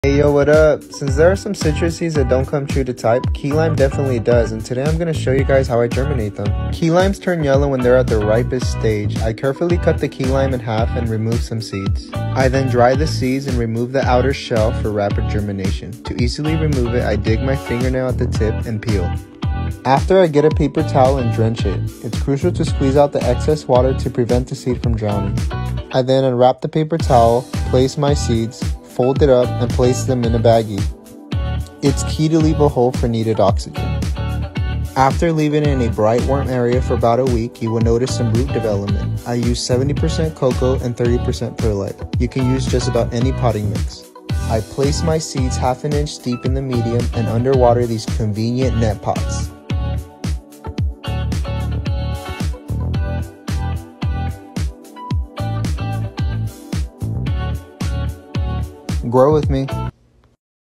How to germinate key-lime Tik Tok
How to germinate key lime! Key lime is one of the citruses that do come true to type meaning the seed will be identical to the parent. 🌱